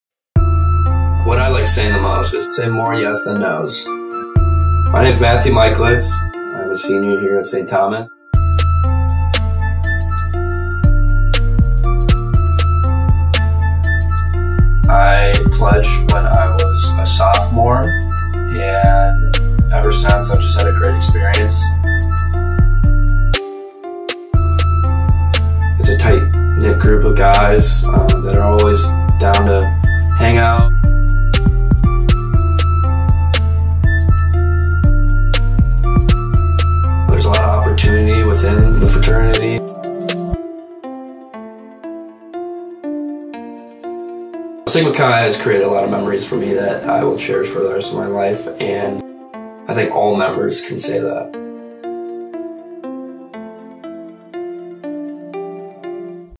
Sigma Chi Instagram Post
Video profile of Mathew Michaletz for the Sigma Chi, Iota Tau Instagram page.